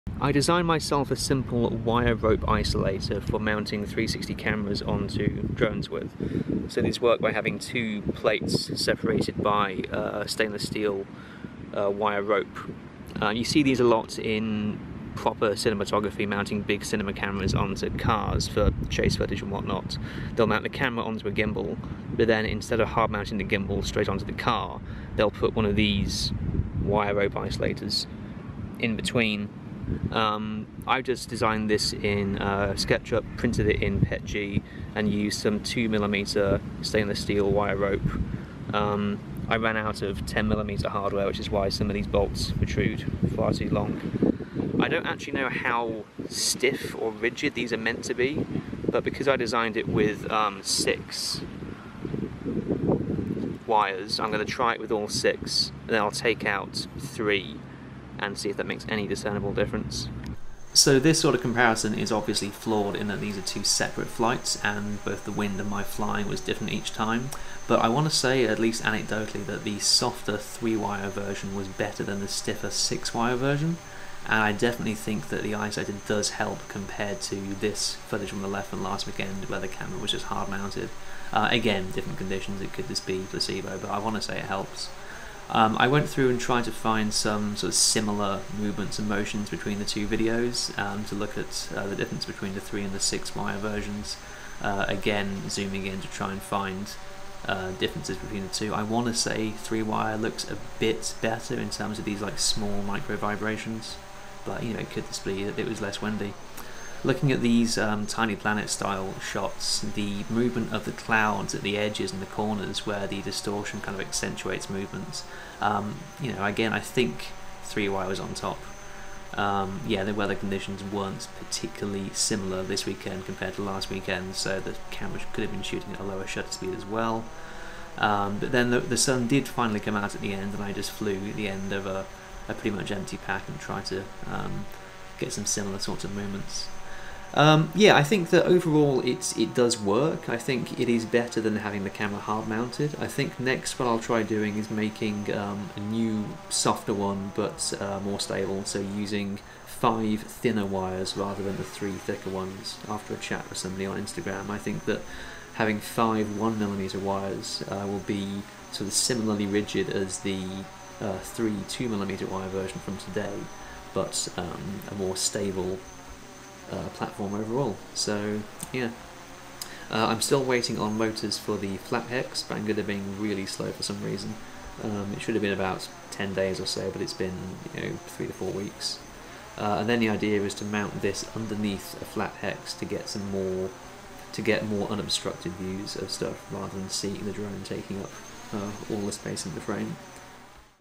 DIY Wire Rope Isolator // 360 Camera // Y6 Everycopter Drone
I'm pretty sure it does help (or at least it doesn't make the footage any worse!) & it was an interesting project! Files will go up on Thingiverse eventually.

Matek F405-STD w/ Arducopter v3.7.0 (dev)
SunnySky X2212-III 1250kv
Gemfan 9x4.5
Racerstar PGA40
Foxeer Predator Mini
Matek VTX-HV
FrSky R9MM
Beitian BN-880